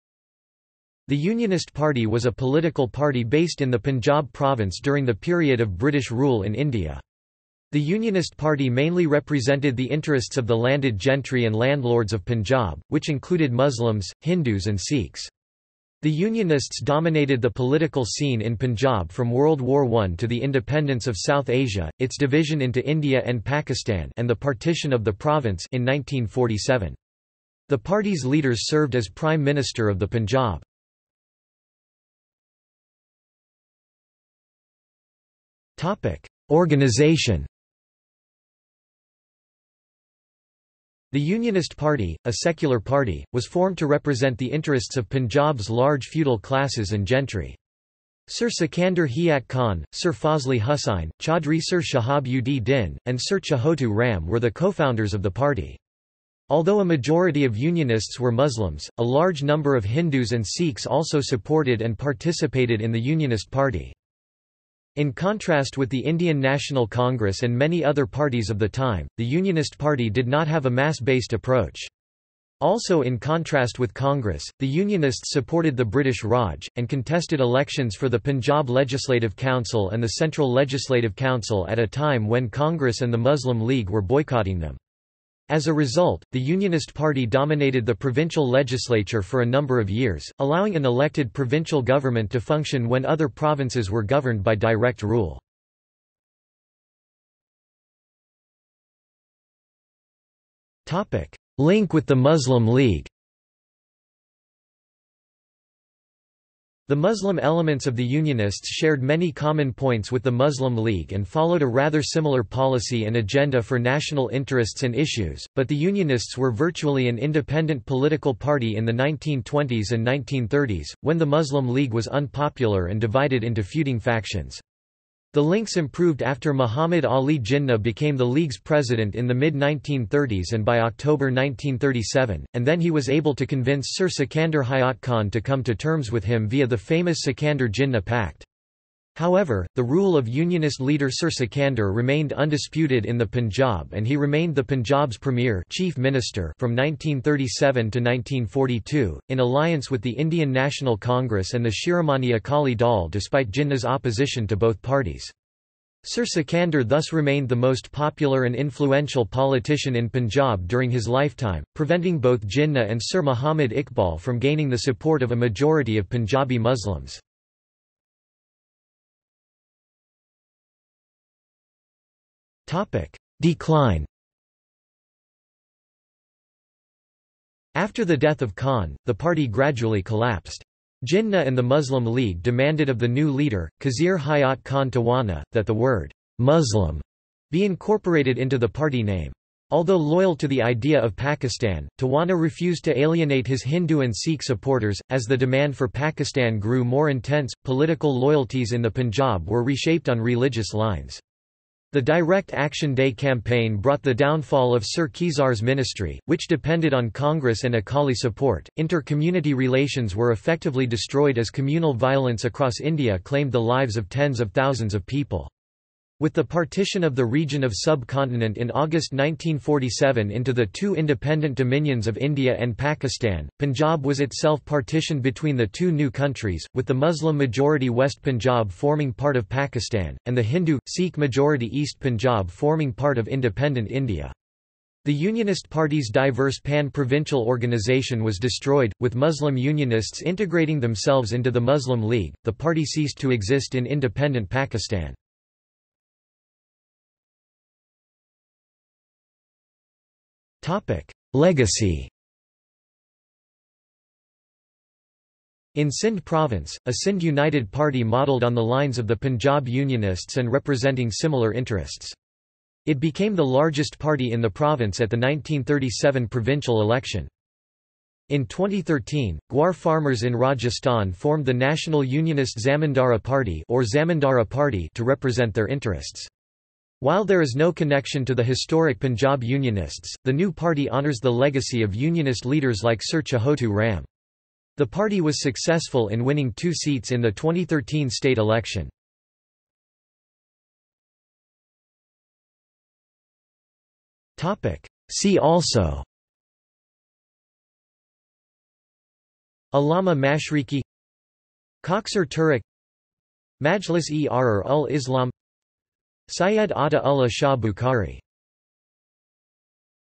Unionist Muslim League | Wikipedia audio article
This is an audio version of the Wikipedia Article:
Unionist Muslim League

00:00:32 1 Organisation
00:01:37 2 Link with the Muslim League
00:02:49 3 Decline
00:04:07 4 Legacy
00:04:50 5 See also

- Socrates

SUMMARY
The Unionist Party was a political party based in the Punjab Province during the period of British rule in India. The Unionist Party mainly represented the interests of the landed gentry and landlords of Punjab, which included Muslims, Hindus and Sikhs. The Unionists dominated the political scene in Punjab from World War I to the independence of South Asia, its division into India and Pakistan (and the partition of the province) in 1947. The party's leaders served as Prime Minister of the Punjab.